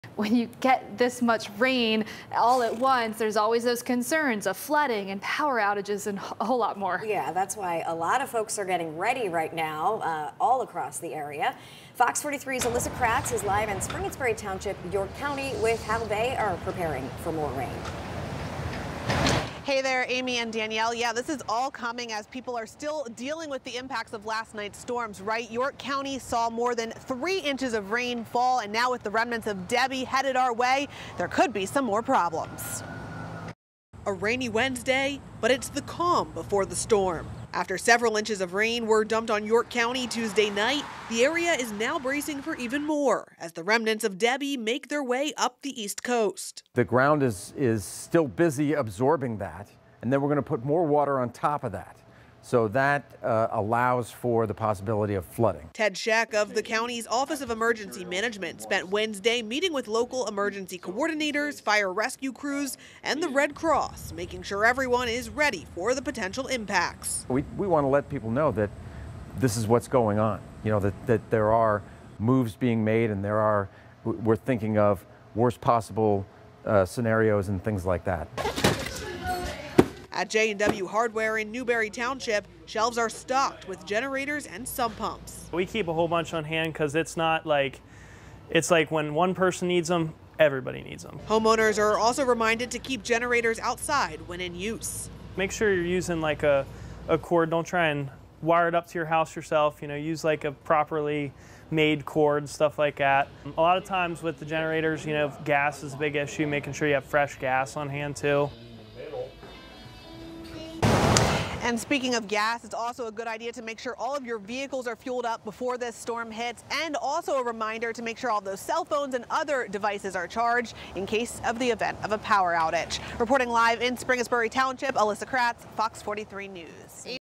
York County prepares for remnants of Tropical Storm Debby
Wednesday was rainy but the calm before the storm.

After several inches of rain were dumped on York County Tuesday night, the area is now bracing for even more, as the remnants of Debby make their way up the east coast.

“The ground is still busy absorbing that and we’re gonna put more water on top of that so that allows for the possibility of flooding," said Ted Czech, public information officer for York County Office of Emergency Management the county's 911 center.

Czech spent Wednesday meeting with local emergency coordinators, fire rescue crews and the Red Cross, making sure everyone was ready for the potential impacts.

WPMT-FOX43's YouTube channel is a hub for the best of FOX43 News and its investigative reports, top headlines, and digital exclusive content: